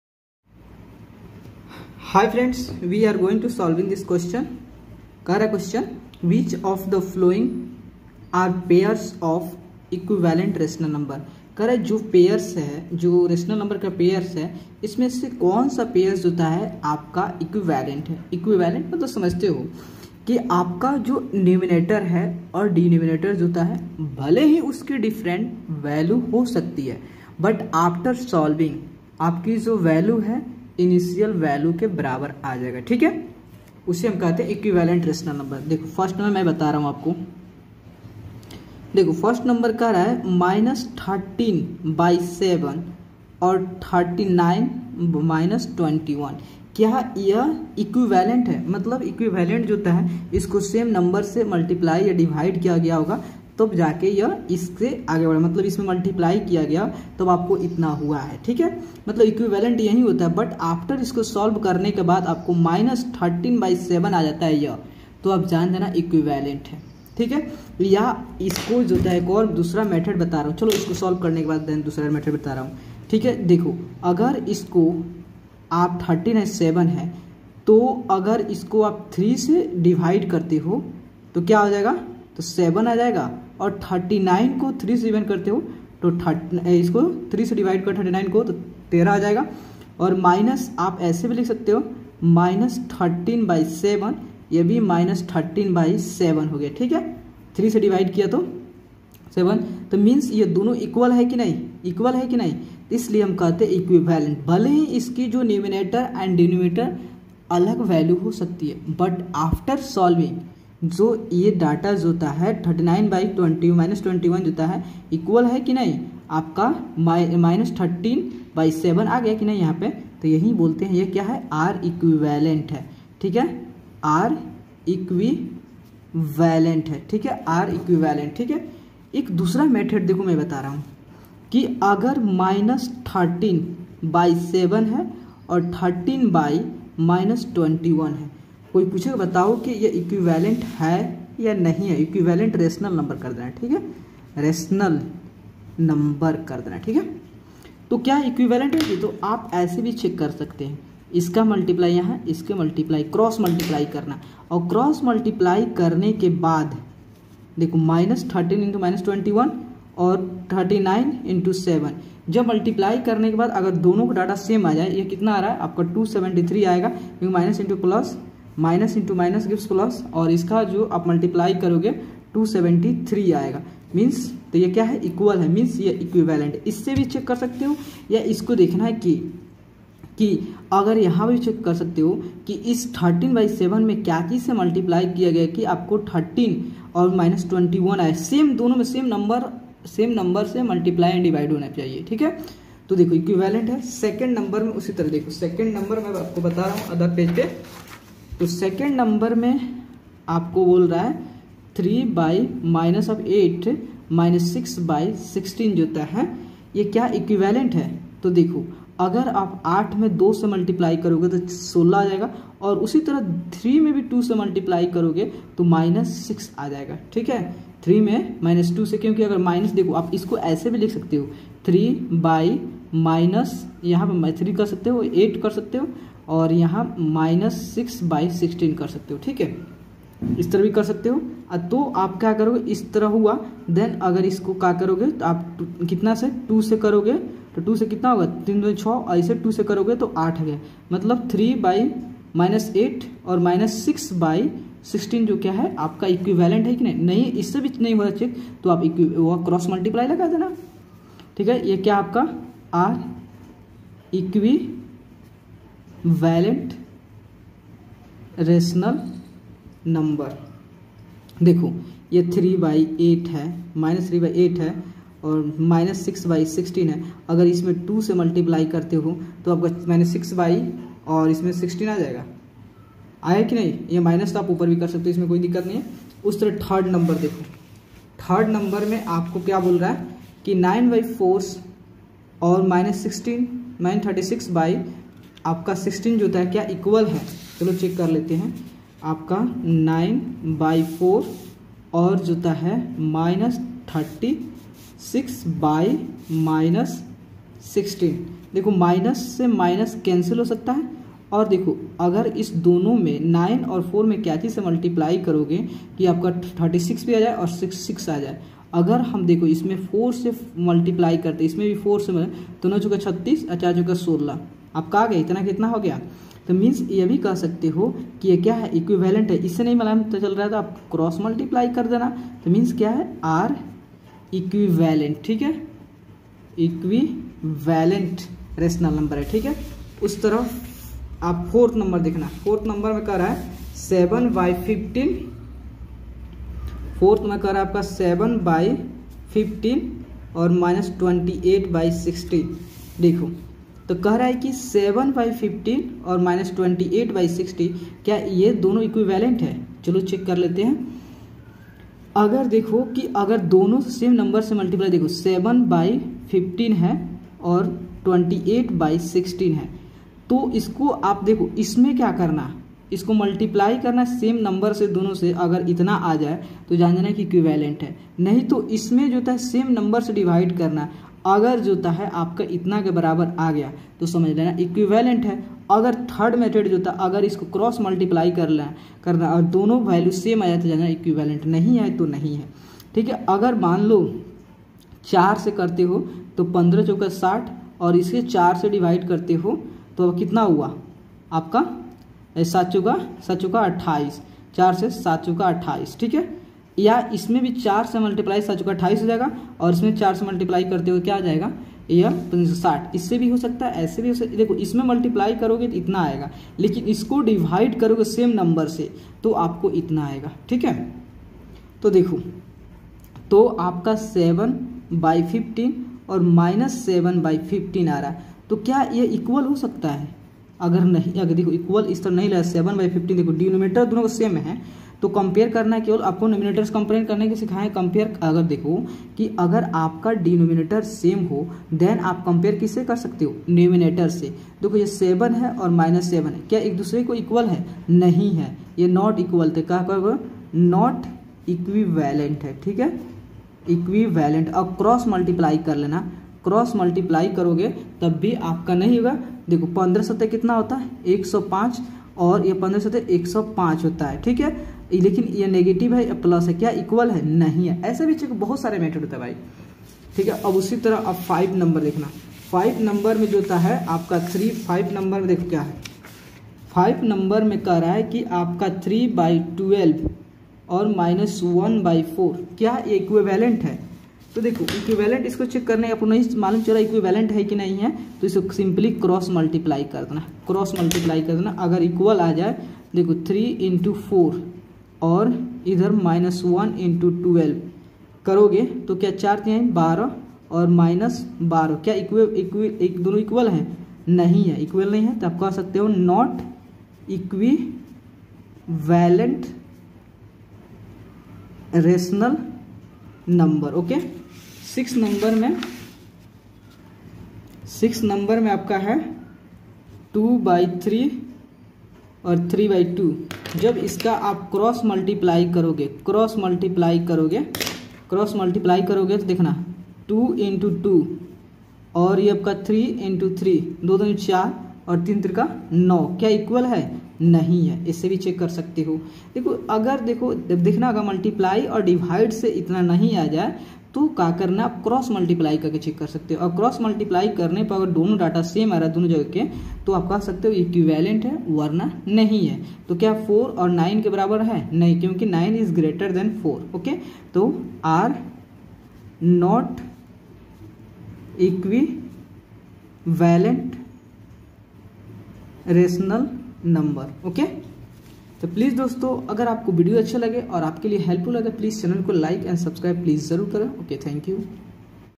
Which of the following are pairs of equivalent rational numbers?1) -13/7 , 39/-21
Which  of the following are pairs of equivalent rational numbers?
1) -13/7  ,  39/-21
2) 3/-8    ,   -6/16
3) 9/4     ,  -36/-16
4) 7/15    ,  -28/60
5) 3/12     , -1/4
6) 2/3      ,  3/2